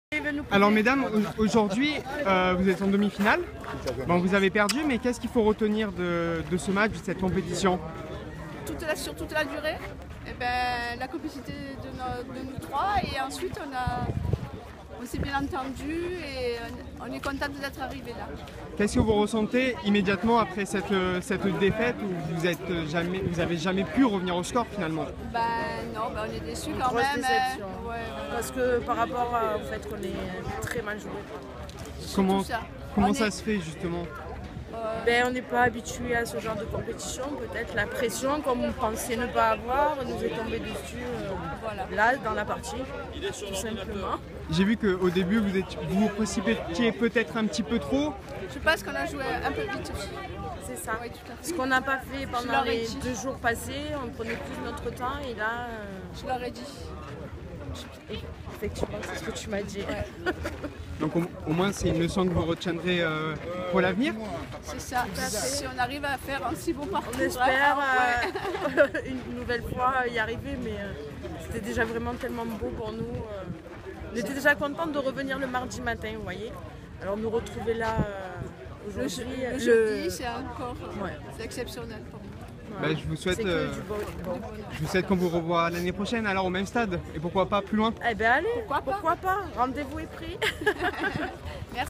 Réaction de la triplette Caron (demi-finale)
La triplette Caron/Girardi/Gruner, représentantes d'Eguilles n'a rien pu faire face à l'équipe Vierjon/Chevalier/Bernard et s'incline 13-2.  Leur réaction sur ce match raté mais aussi sur leur fabuleux parcours dans ce Mondial.